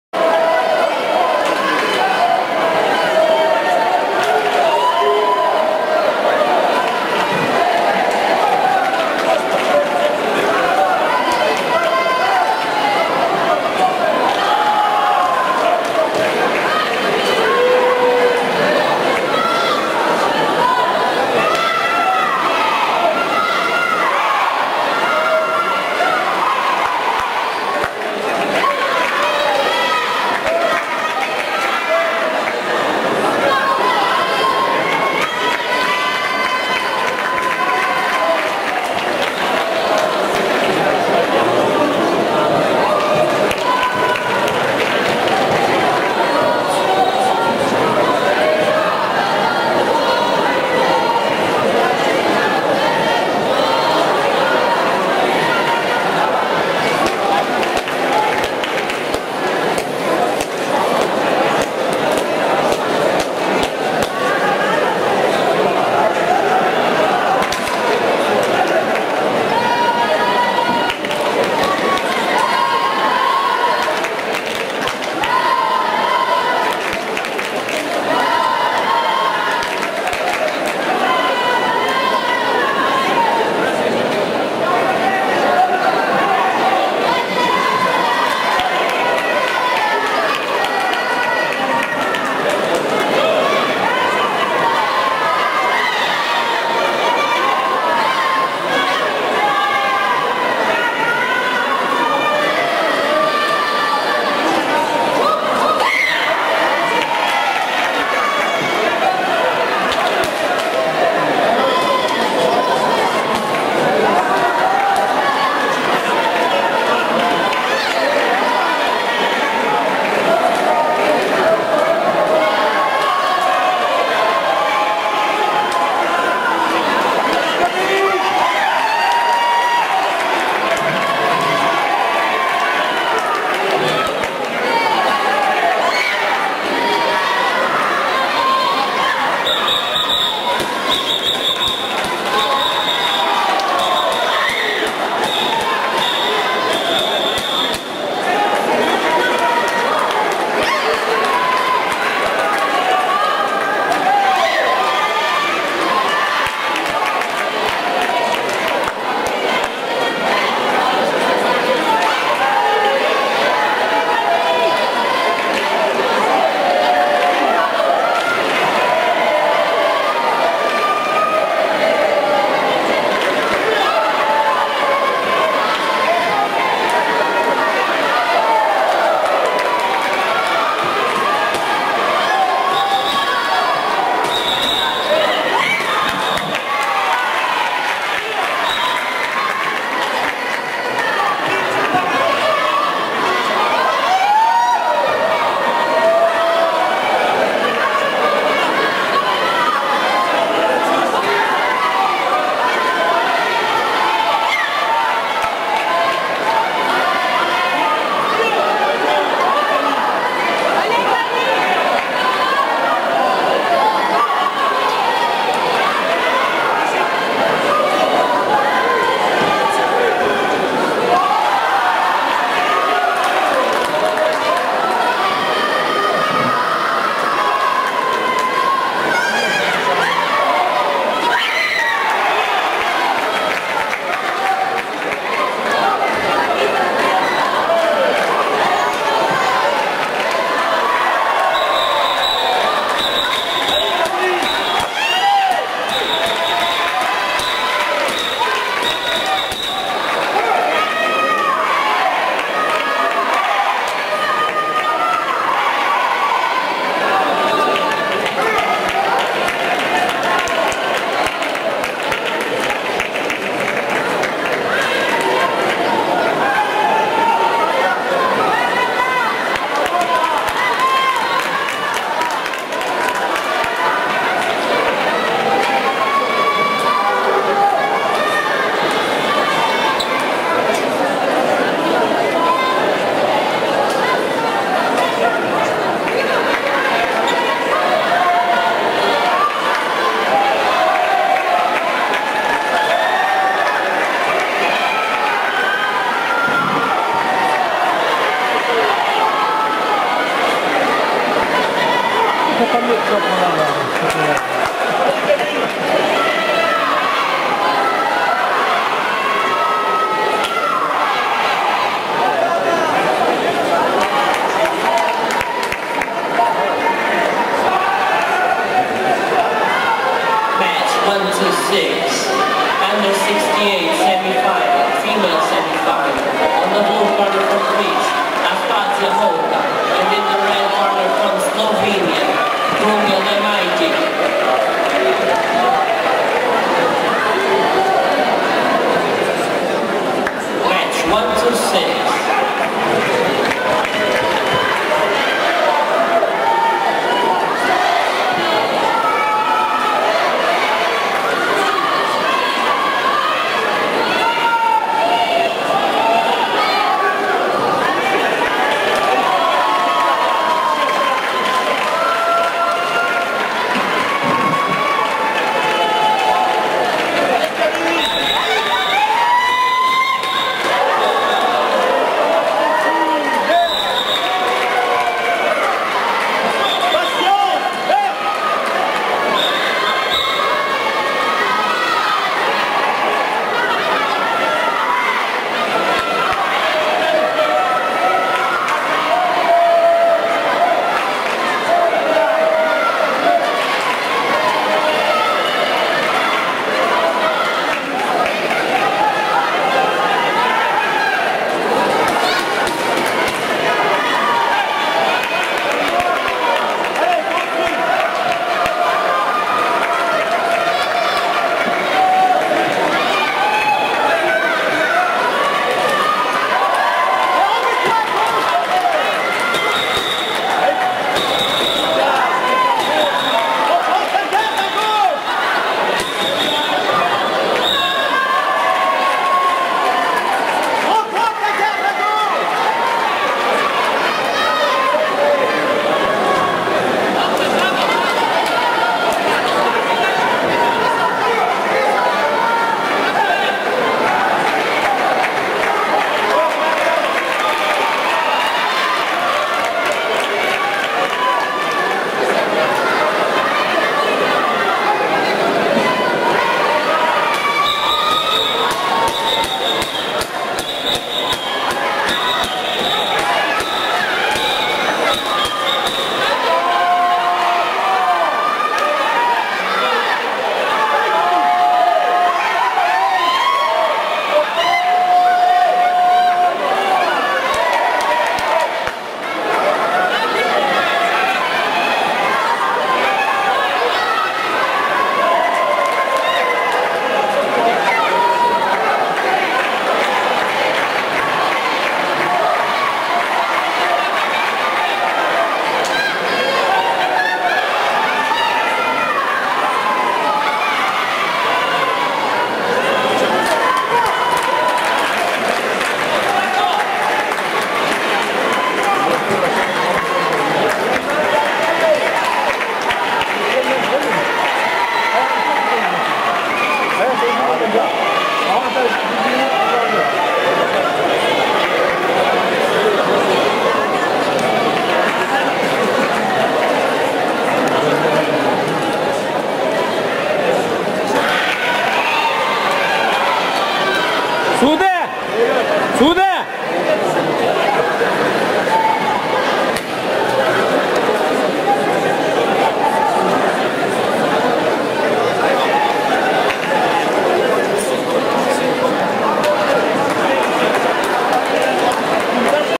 63kg Quarterfinal Sude BULUT (TUR) vs (FRA) PRODEAU Camille
19th Europen Junior Taekwondo Championships 2013.
25-28 September 2013 - Portugal